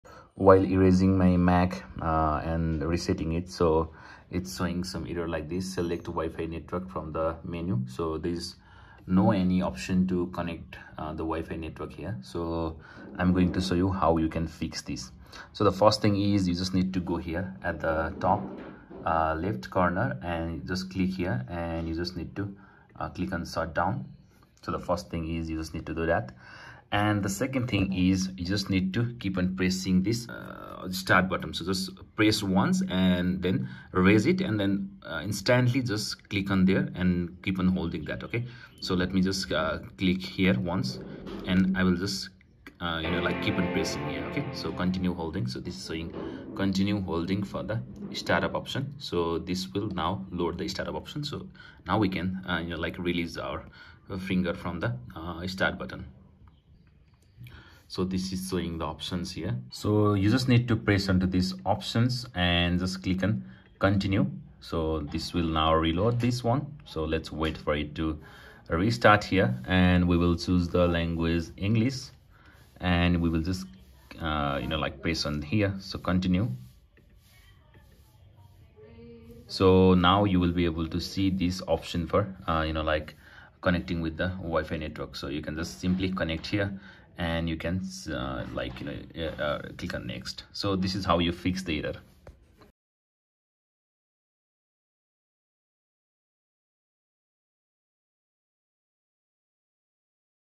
How To Fix No Wi-Fi Network From The Menu on Macbook (M1/ M2) (Activate Mac)
In this video I have shown how you can fix the error No Wi-Fi Network from the Menu in Macbook M1 / M2 while resetting or erasing all the data.